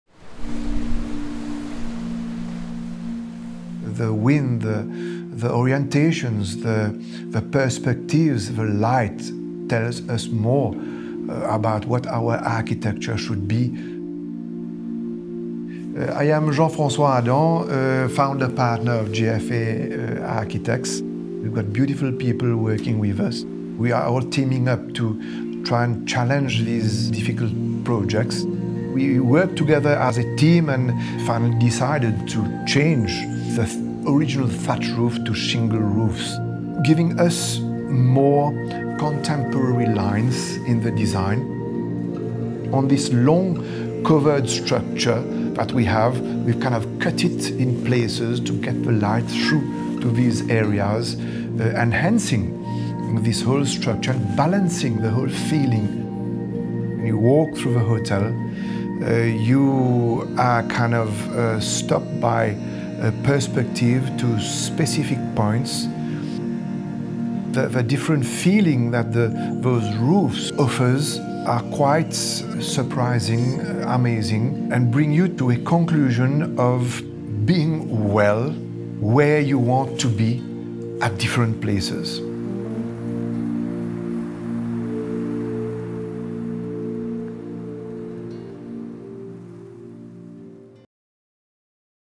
Dans les coulisses du nouveau LUX* Belle Mare - Ep 4 , L'architecture (FR subtitles)
Le quatrième et dernier épisode de notre mini-série, "The Making of", vous invite à explorer le concept architectural créé par JFA Architects. L'architecte mauricien, Jean-François Adam, nous éclaire sur les défis de concilier modernité et préservation du patrimoine, et à travailler avec les ingénieurs les plus créatifs de l'île.